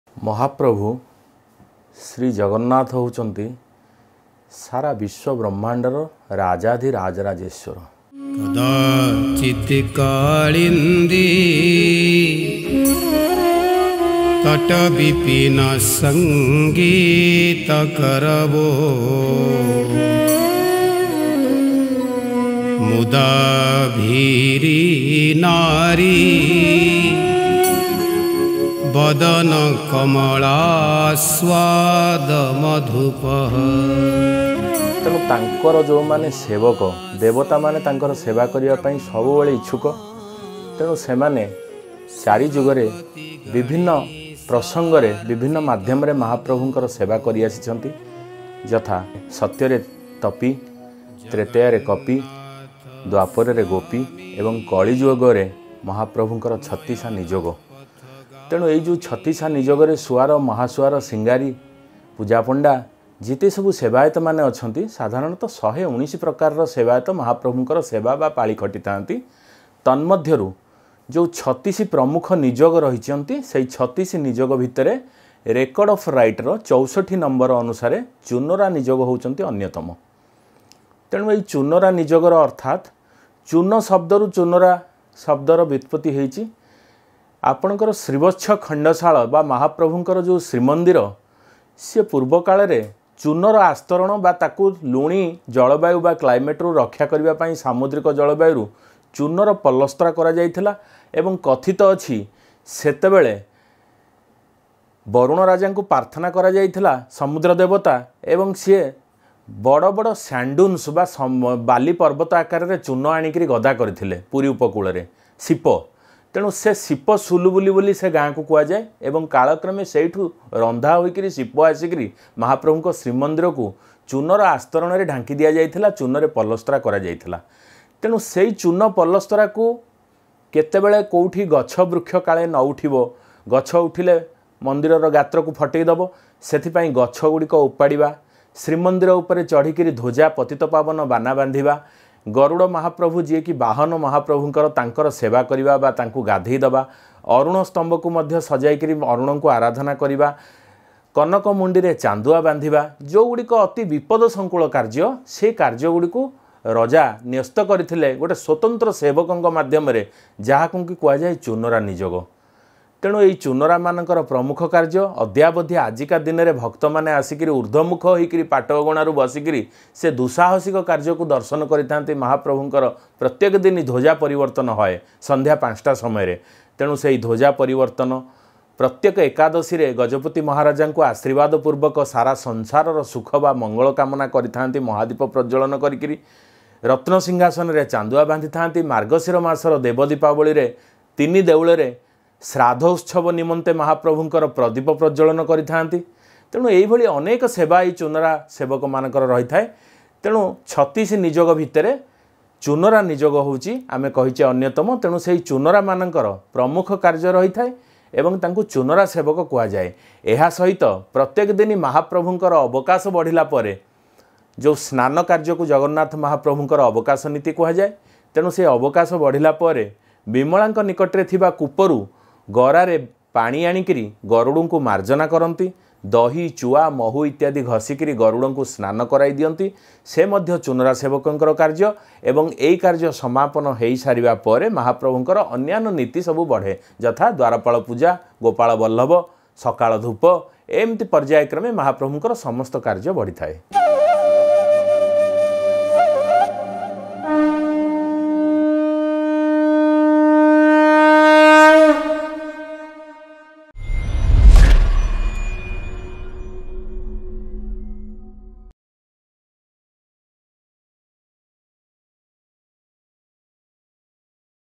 ଚୁନରା ସେବକ | Chunara Sebaka | Srimandira Seba O Sebayat - 64 | Dr. Sarat Chandra Mohanty | Odisha365
In this series oif Srimandira Seba o Sebayat we will try to bring information about all Sebayat of Puri Jagannath Temple.
Chatisha Nijoga

Seba O Sebayat